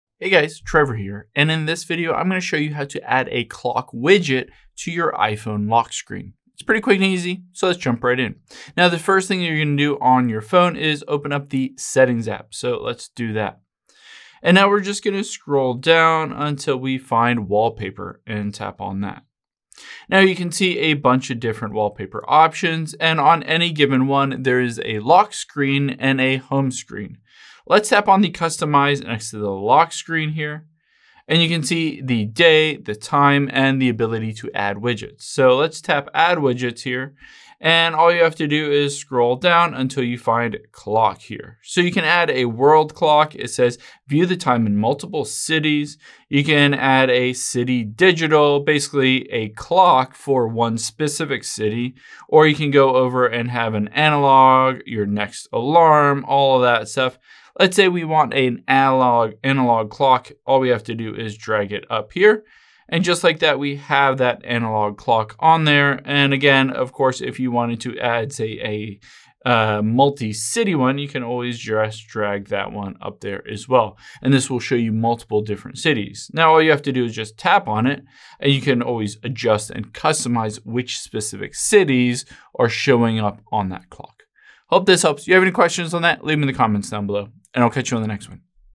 How To Add Clock Widget To iPhone Lock Screen
Let's add a clock widget to your iPhone Lock Screen so that you can easily see an old school analog version.

Cool iPhone Facts:
- Every single iPhone ad shows the time as exactly 9:41